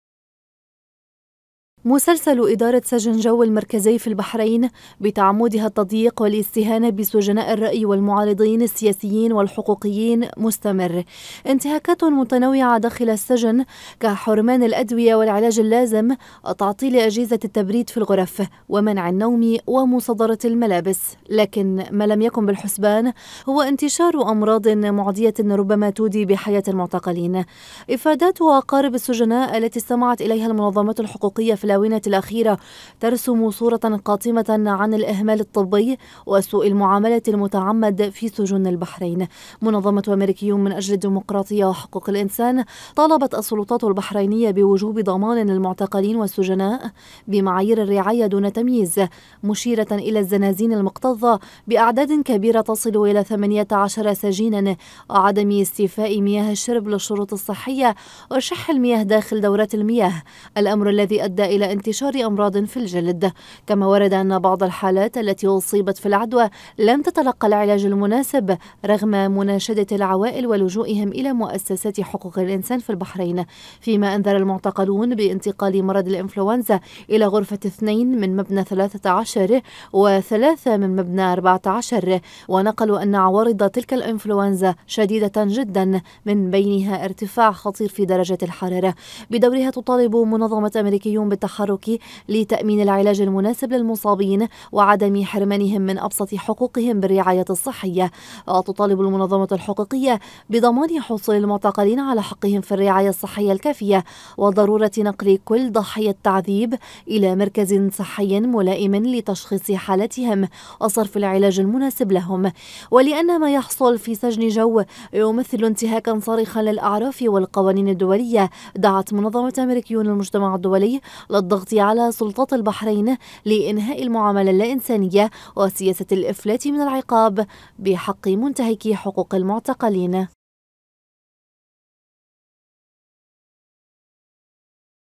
تقرير متلفز: معتقلو سجن جو يعانون من انتهاكات مستمرة
يتعرض آلاف المعتقلين البحرينيين في سجن جو المركزي الى انتهاكات مستمرة من قبل سجانيهم، تتنوع بين أساليب التعذيب الجسدي والنفسي وسوء المعاملة وإنتهاك الكرامة الإنسانية للمعتقلين.

وترفع منظمات حقوقيّة بحرينية وغربية الصوت عاليا للمطالبة بوضع حد لنهج السلطات بحق المواطنبن والمعتقلين، فيما يستمر غض الطرف من قبل الحكومات الغربية الذي يصل إلى حد تقديم الدعم المعنوي والسياسي والتسليحي للسلطات.